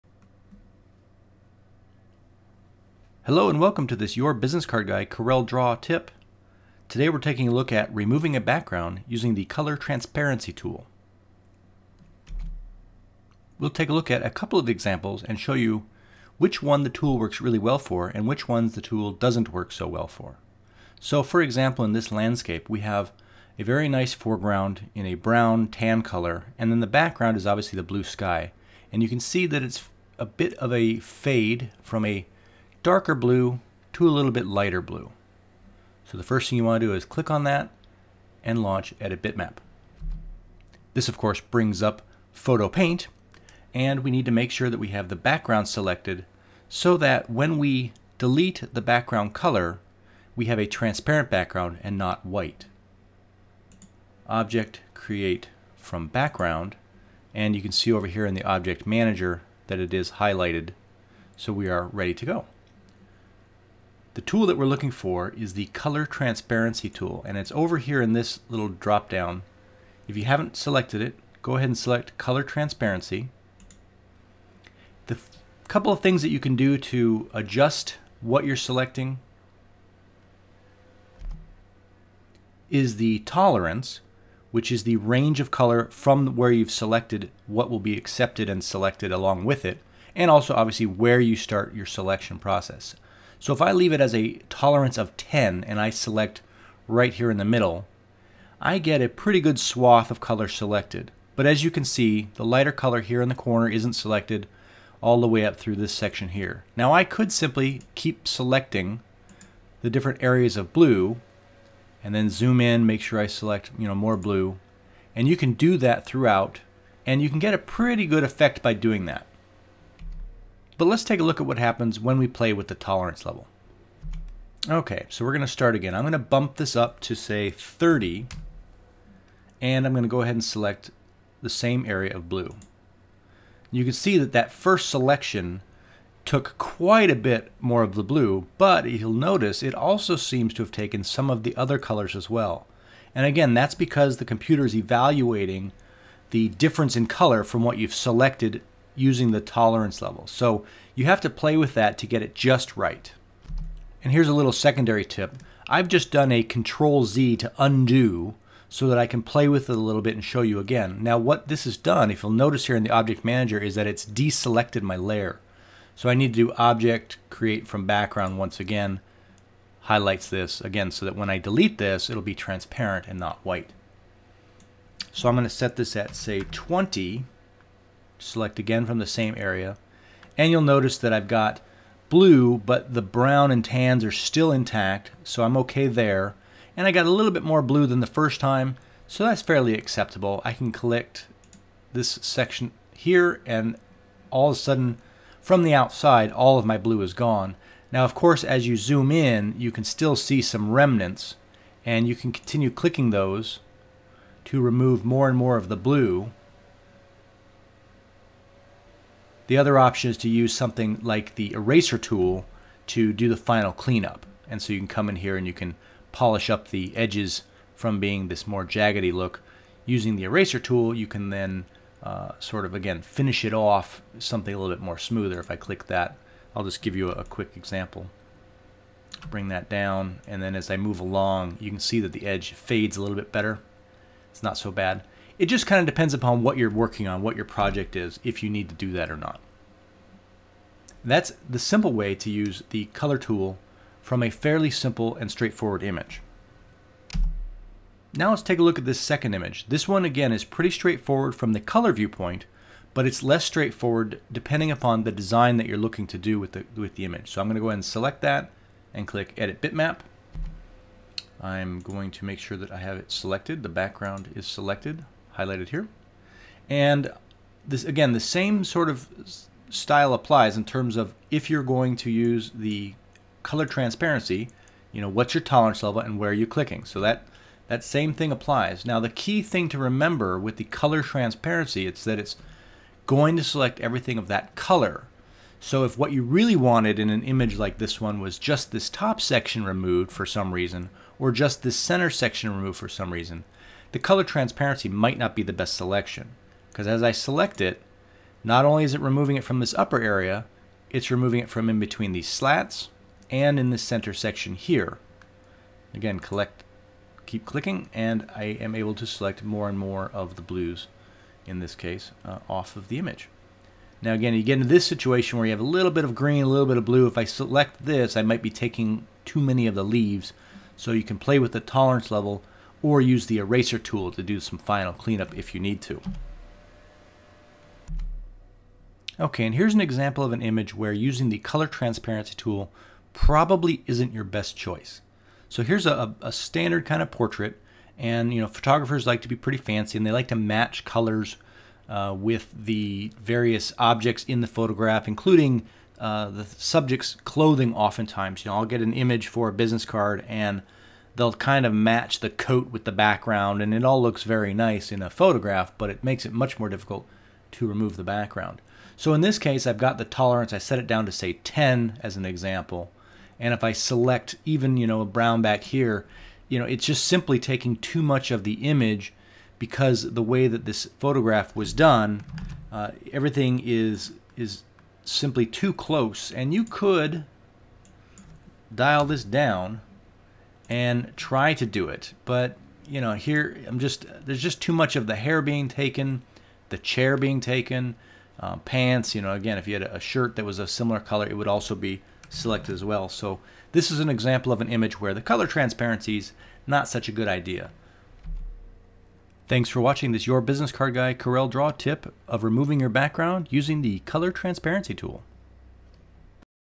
COREL DRAW TUTORIAL - REMOVING A BACKGROUND - COLOR TRANSPARENCY TOOL
Coreldraw (Corel Photo Paint) have many ways to select and remove a background. This tutorial covers using the color transparency tool.